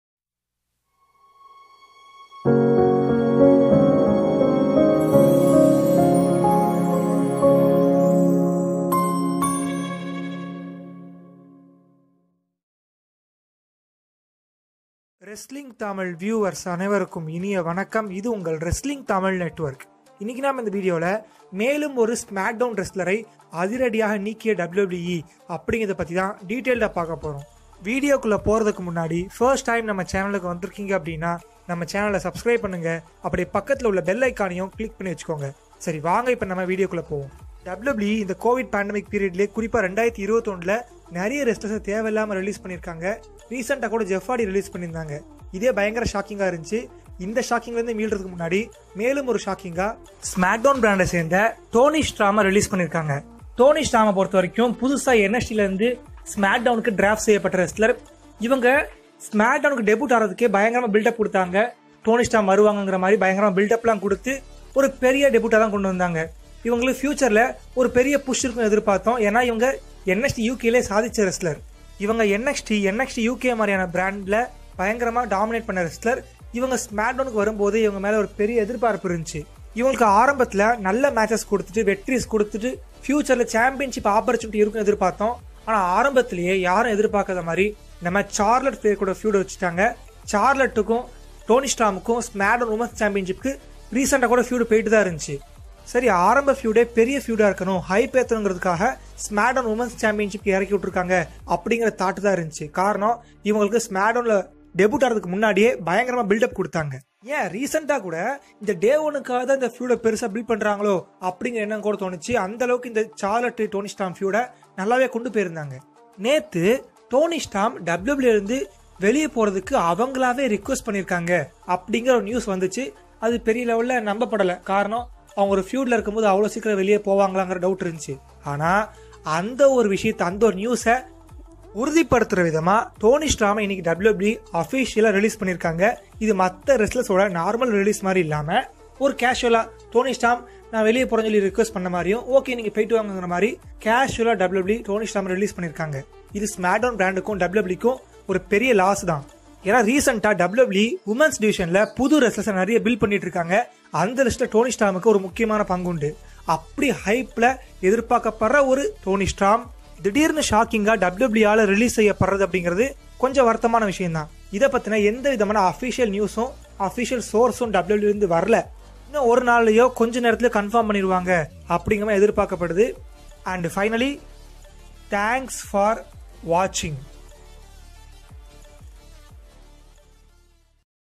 மேலும் ஒரு Smackdown Wrestler ஐ அதிரடியாக நீக்கிய (வெளியேற்றிய) WWE // Wrestling Tamil Network
WWE Releases Toni Storm Full Explain Tamil